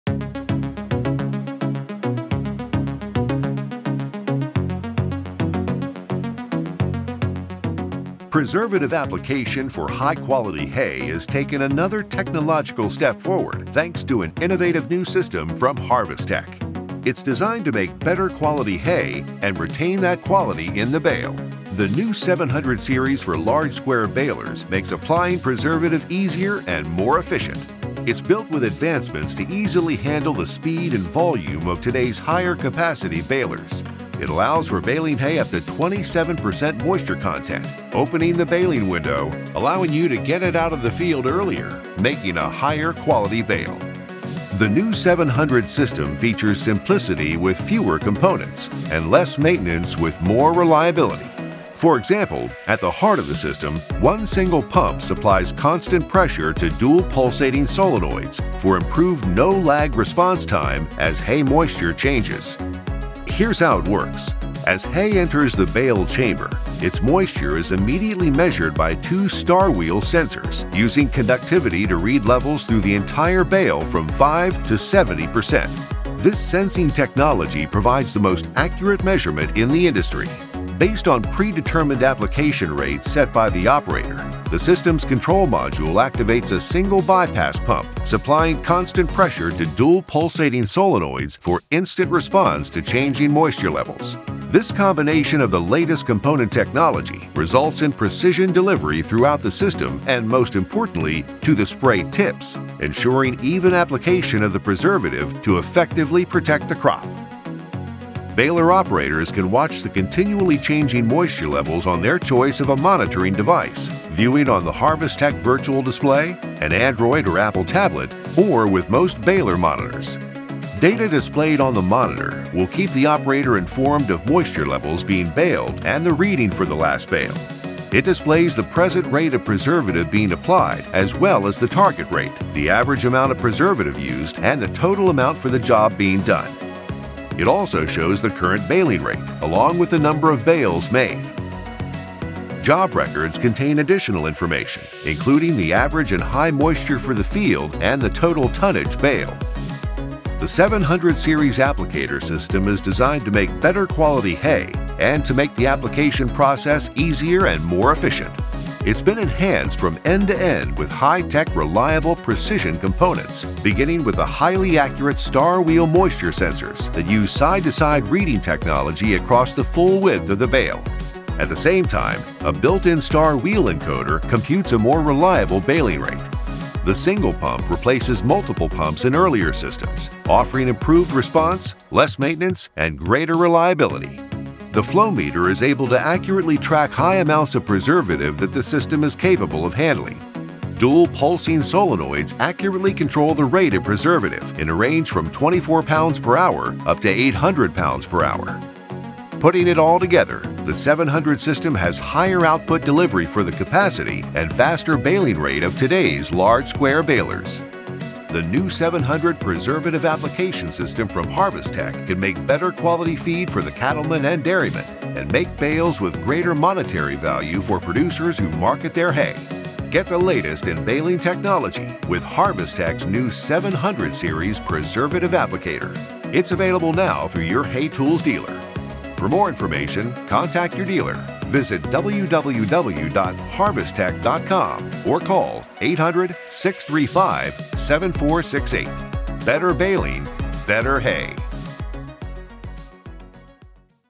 Harvest Tec 700 Series Automatic Preservative Applicator System Baler Animation
See the brand new Harvest Tec 700 Series Automatic Preservative Applicator in action on a large square baler animation! Highlighting the new simple easy to use design and precision operating equipment.